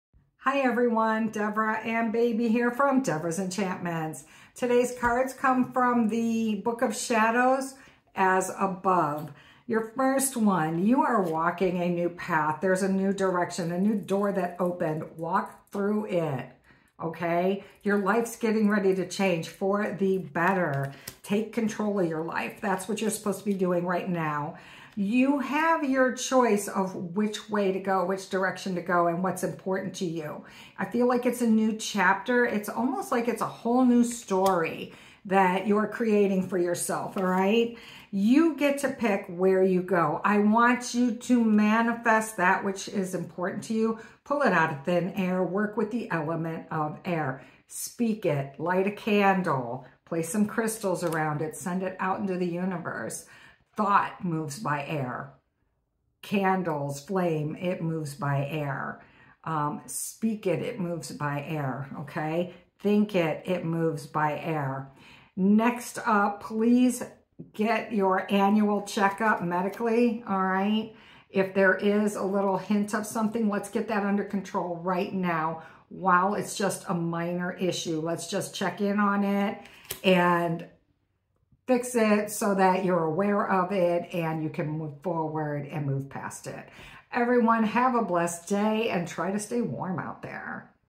A new door opens. Time to take the path less traveled by others.
Today's cards comes from the Book of Shadow Tarot.  Today I am reading the 1st of two decks called "as above".